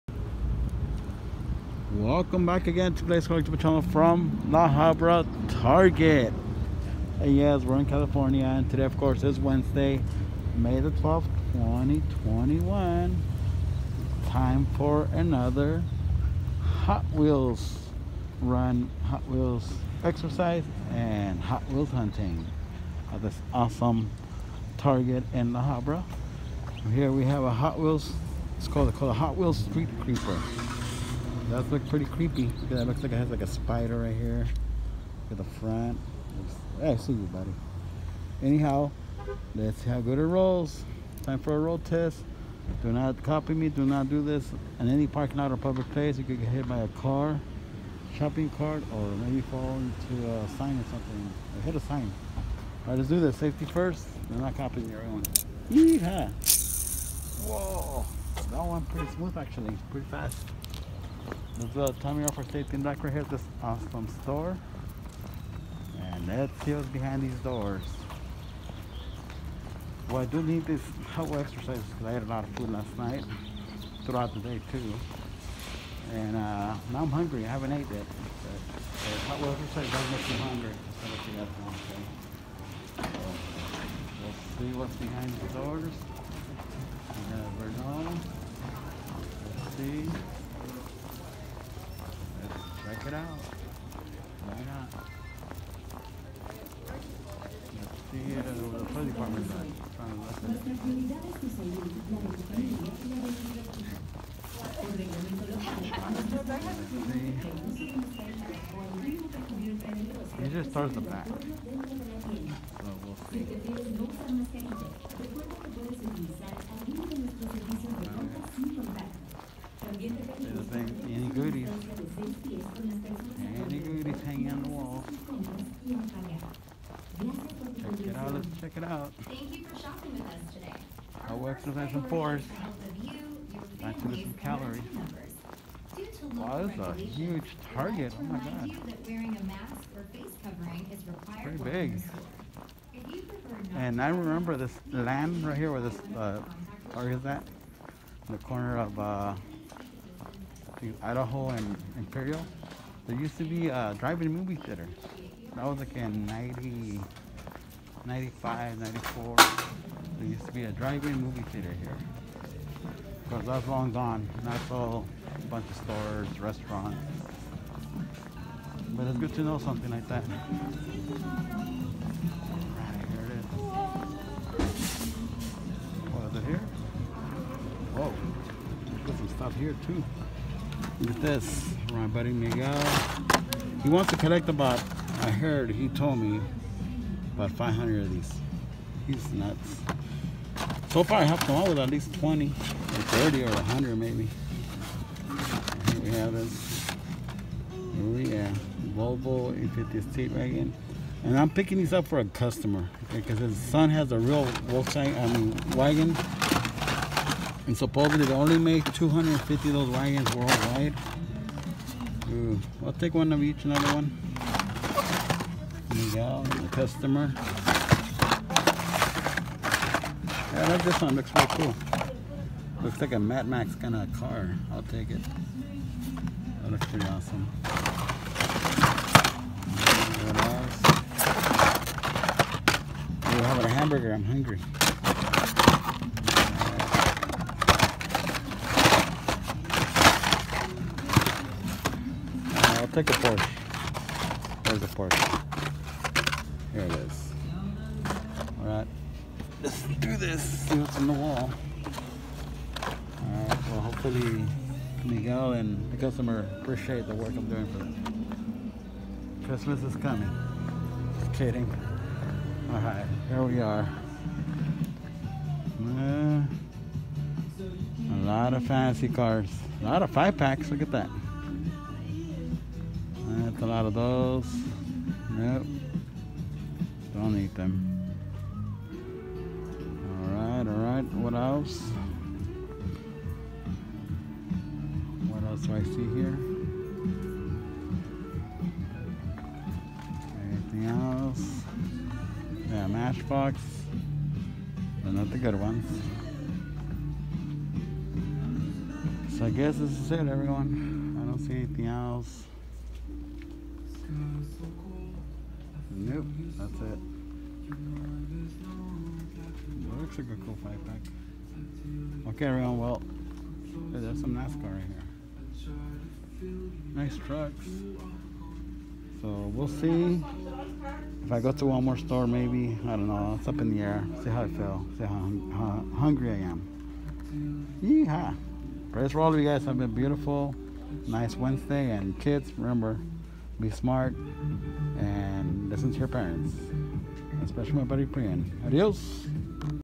Playdays Collectibles Wednesday morning Hotwheels hunting at Target!. 5.12.21
Let’s try to get Hotwheels lucky at Target!. Playdays Collectibles is now open!. Bring your mask 😷 and gloves 🧤. We are all equal!. Safety first!. Please keep using all safety precautions at all times!. Open 7 days a week. Thank you for your business and support. Don’t forget to visit my YouTube channel